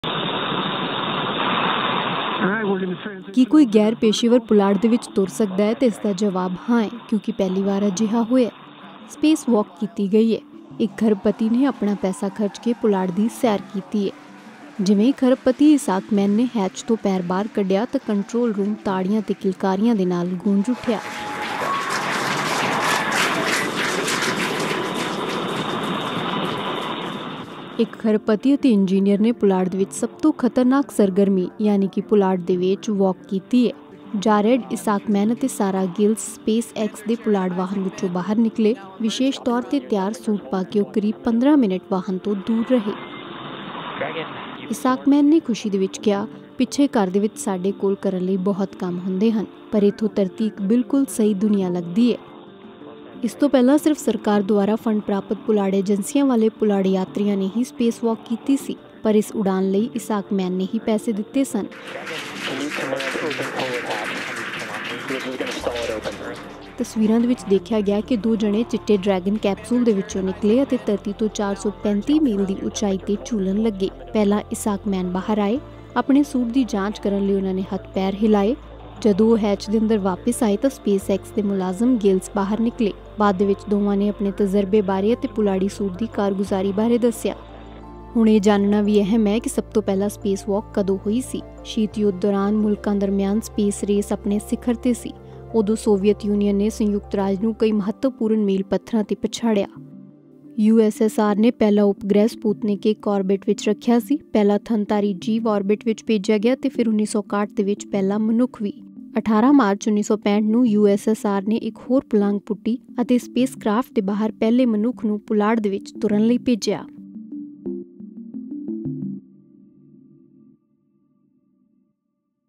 Spacewalk: ਪਹਿਲੀ ਵਾਰ ਕੋਈ ਗੈਰ ਪੇਸ਼ੇਵਰ ਆਪਣੇ ਪੈਸੇ ਖਰਚ ਕੇ ਪੁਲਾੜ ਵਿੱਚ ਤੁਰਿਆ | 𝐁𝐁𝐂 𝐏𝐔𝐍𝐉𝐀𝐁𝐈
ਕੀ ਕੋਈ ਗੈਰ ਪੇਸ਼ੇਵਰ ਪੁਲਾੜ ਵਿੱਚ ਤੁਰ ਸਕਦਾ ਹੈ ਤਾਂ ਇਸ ਦਾ ਜਵਾਬ ਹਾਂ ਹੈ ਕਿਉਂਕਿ ਪਹਿਲੀ ਵਾਰ ਅਜਿਹਾ ਹੋਇਆ, ਸਪੇਸ ਵਾਕ ਕੀਤੀ ਗਈ ਹੈ। ਇੱਕ ਖ਼ਰਬਪਤੀ ਨੇ ਆਪਣਾ ਪੈਸਾ ਖਰਚ ਕੇ ਪੁਲਾੜ ਵਿੱਚ ਤੁਰਿਆ ਹੈ।ਜਿਵੇਂ ਹੀ ਖਰਬਪਤੀ ਇਸਾਕਮੈਨ ਨੇ ਹੈਚ ਤੋਂ ਪੈਰ ਬਾਹਰ ਕੱਢਿਆ ਤਾਂ ਕੰਟਰੋਲ ਰੂਮ ਤਾੜੀਆਂ ਤੇ ਕਿਲਕਾਰੀਆਂ ਨਾਲ ਗੂੰਜ ਉੱਠਿਆ।
ਇੱਕ ਖਰਬਪਤੀ ਅਤੇ ਇੰਜੀਨੀਅਰ ਨੇ ਪੁਲਾੜ ਵਿੱਚ ਸਭ ਤੋਂ ਖ਼ਤਰਨਾਕ ਸਰਗਰਮੀ ਯਾਨਿ ਪੁਲਾੜ ਵਿੱਚ ਵਾਕ ਕੀਤੀ ਹੈ , ਇਸਾਕਮੈਨ ਅਤੇ ਸਾਰ੍ਹਾ ਗਿਲਸ ਸਪੇਸ-ਐਕਸ ਦੇ ਪੁਲਾੜ ਵਾਹਨ ਵਿੱਚੋਂ ਬਾਹਰ ਨਿਕਲੇ। ਵਿਸ਼ੇਸ਼ ਤੌਰ ਤੇ ਤਿਆਰ ਸੂਟ ਪਾ ਕੇ ਉਹ ਕਰੀਬ 15 ਮਿੰਟ ਵਾਹਨ ਤੋਂ ਦੂਰ ਰਹੇ।